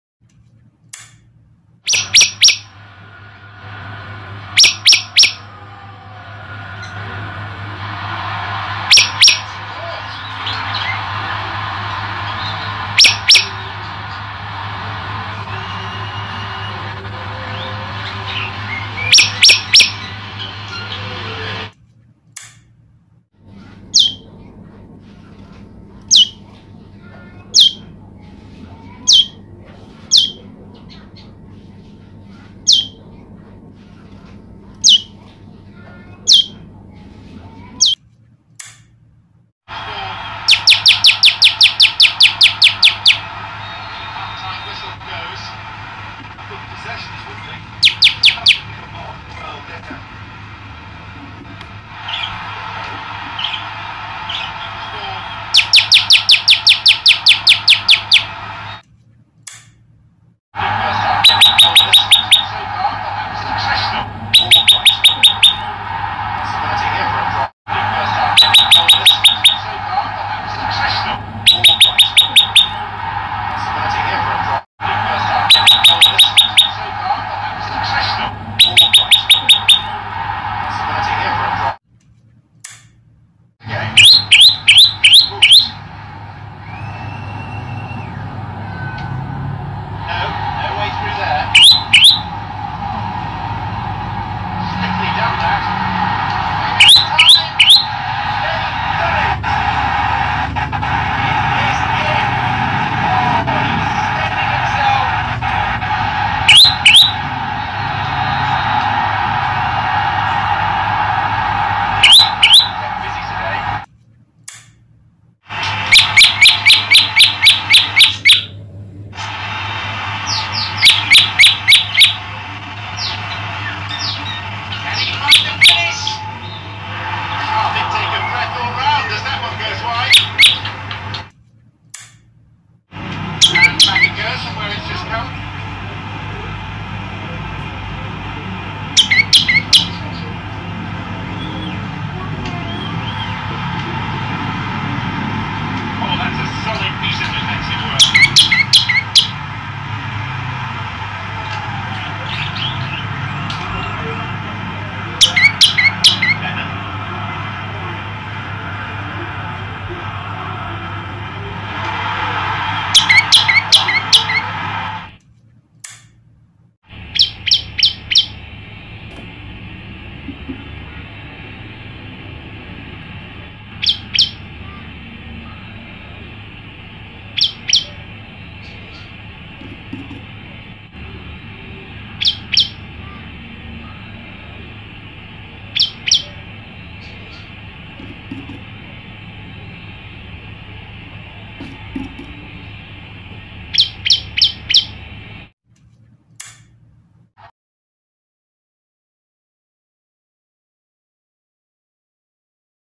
9 variasi prenjak lumut jantan gacor
Kali ini channel euweuh gawe upload video burung prenjak lumut gacor 9 kecupan mesra dari tanteu imut 🤣 atau 9 variasi suara prenjak lumut janda eh jantan maksudnya ..
video burung prenjak lumut jantan atau prenjak bawah jantan ini sudah pernah saya upload di channel ini jauh jauh hari dan sekarang saya upload lagi ..
mengenang burung ciang ciang jantan ini yang sudah saya lepas liarkan di kampung halaman saya ..